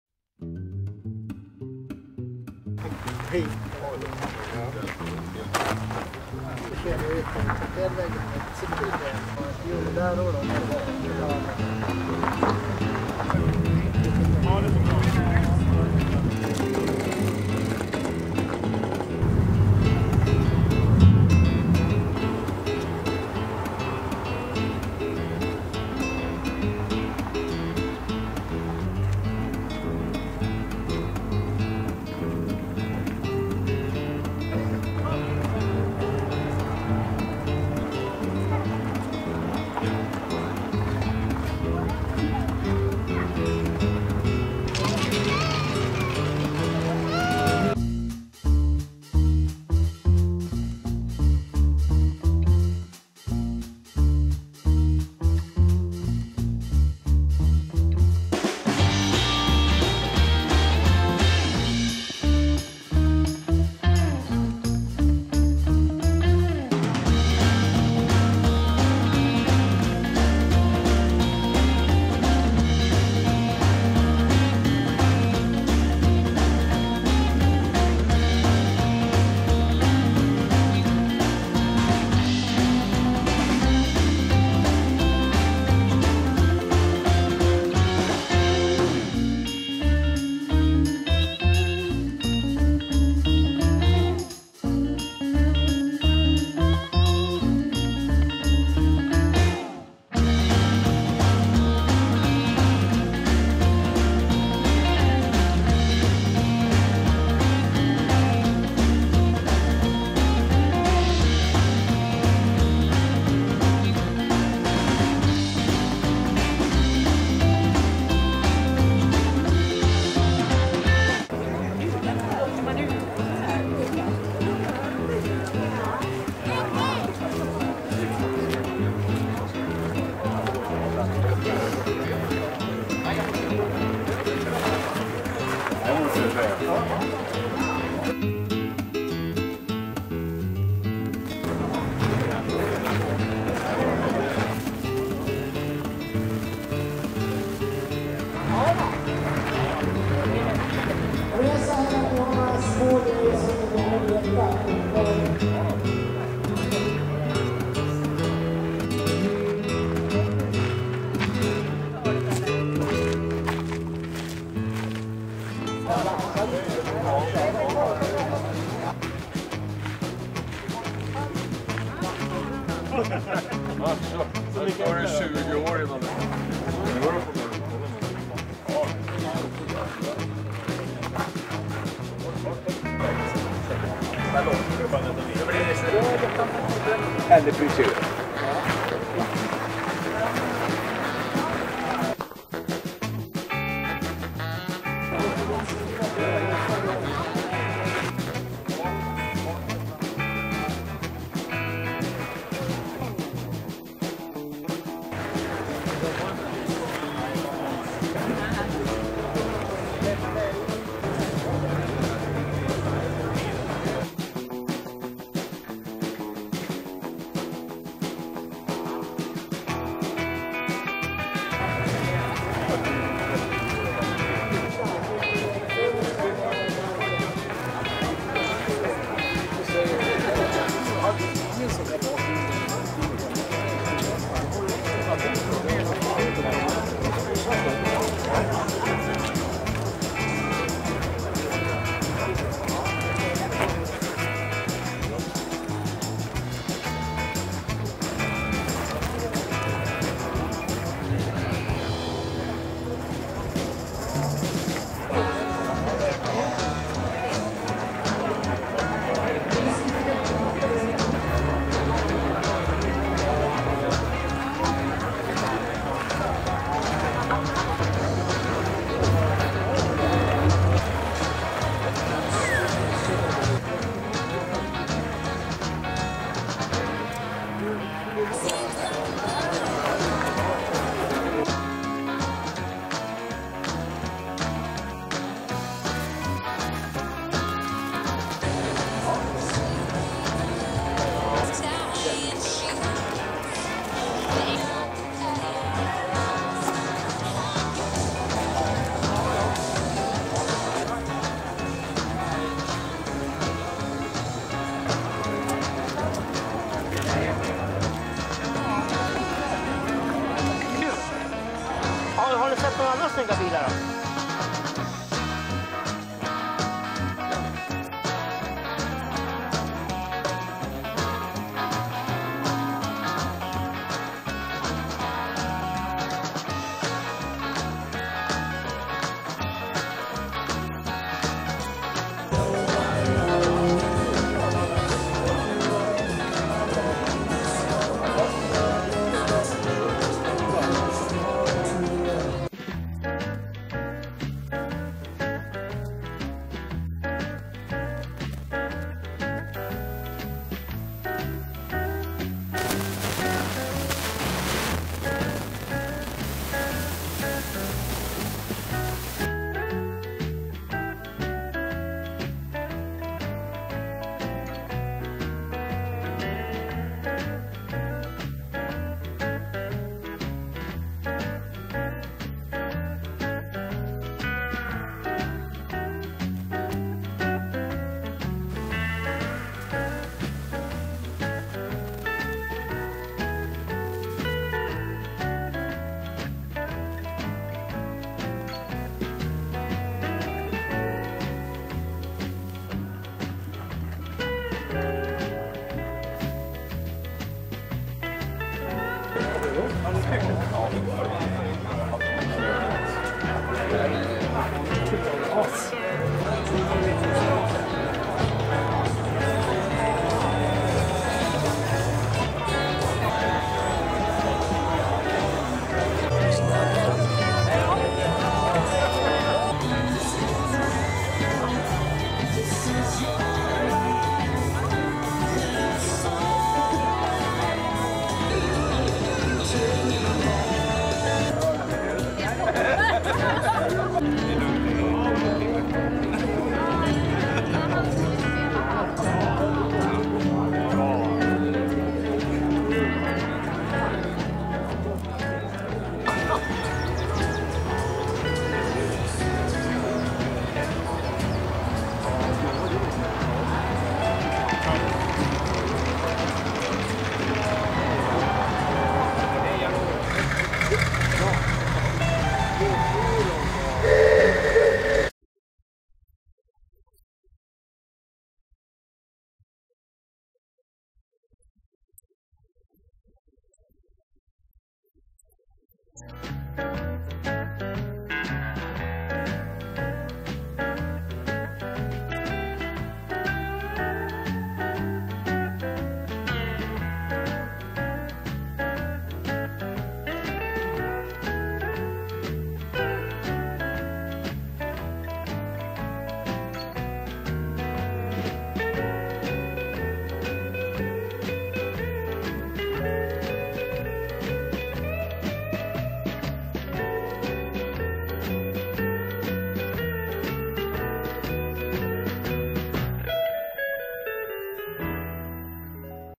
SSRA Nats - Hedemora - 2023 (Hot Rods in Event)
0:00 Roddar på parkeringen (Rods on the parking)
0:45 Utställningen från ovan (The Exhibition from drone)
1:55 Utställningen (The Exhibition)